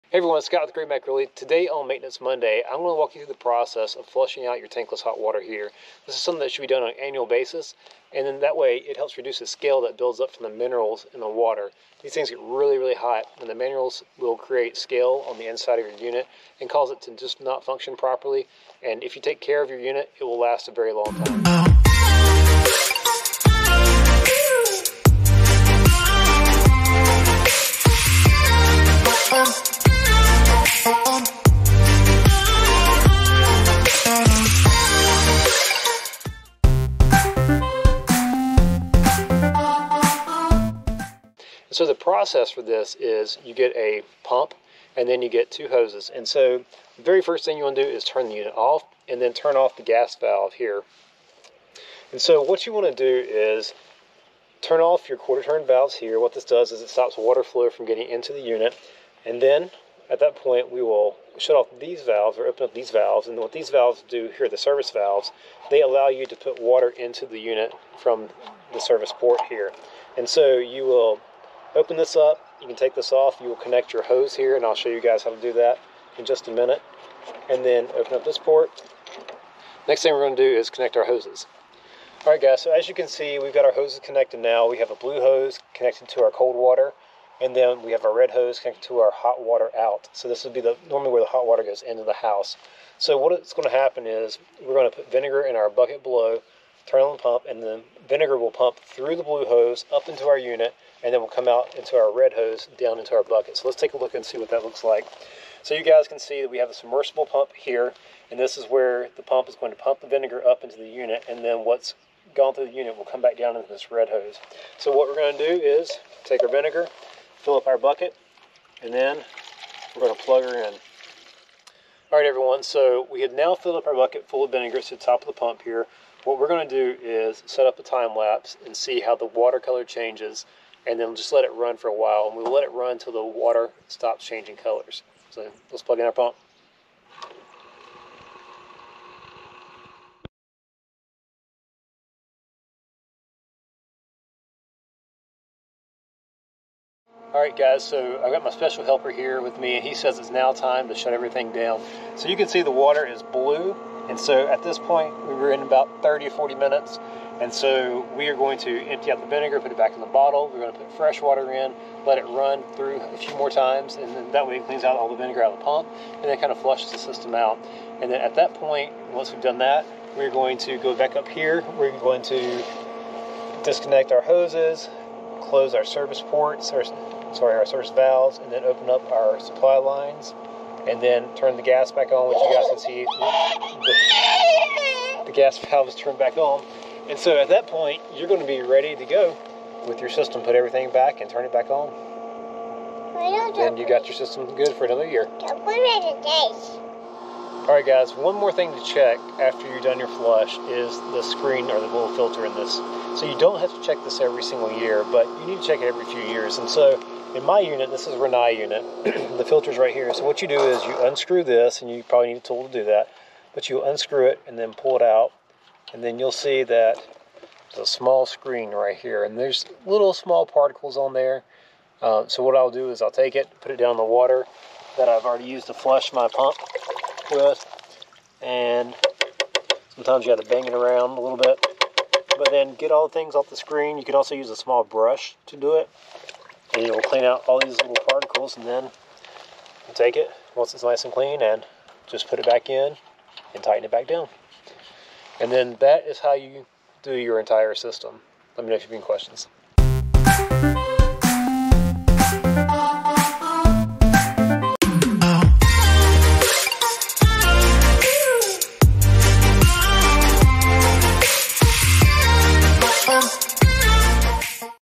How to flush a tankless hot water heater
Tankless hot water heaters should be maintained on a annual basis to insure that they will last you for many years.  This video walks you through the steps to service your tankless hot water heater each year for fractions of what it cost to have a plumber do it!

Link to the pump
Link to the hoses

GrayMac Realty by eXp Realty
"Expect More"

Scott Moore
Realtor
Oak Ridge, NC